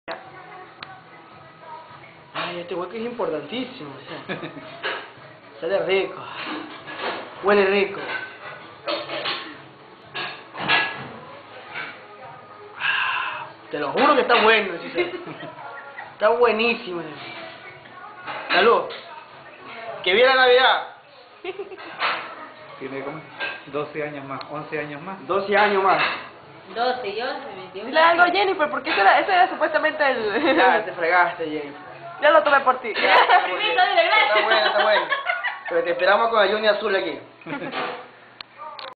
NAVIDAD 2010
Mi primo César probando y deleitando el wisky que mi papá por tantos años lo guardo para mi hermana Jeniffer